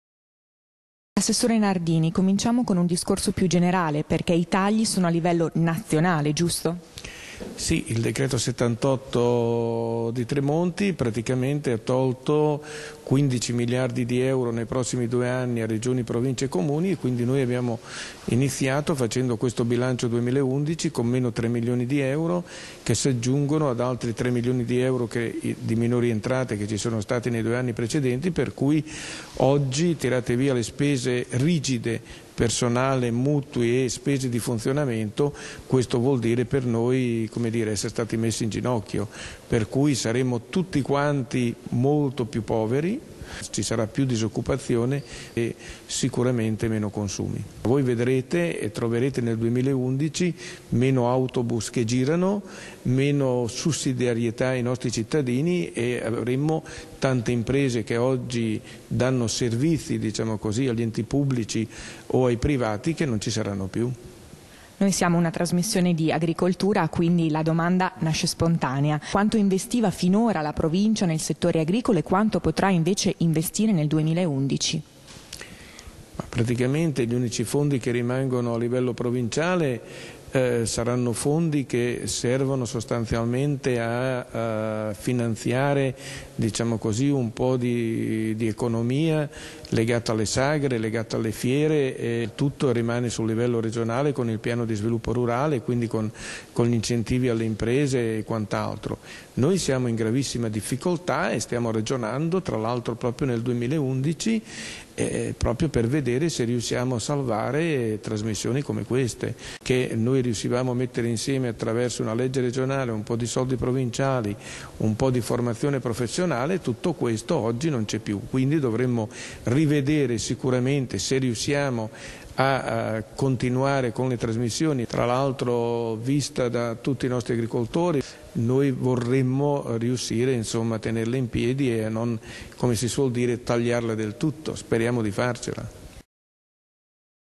DAVIDE NARDINI
Nella seconda parte siamo stati alla presentazione del bilancio preventivo 2011 della Provincia e abbiamo cercato di capire che cosa succederà per il settore agricolo. Ecco l'intervista all'assessore al Bilancio della Provincia di Ferrara Davide Nardini.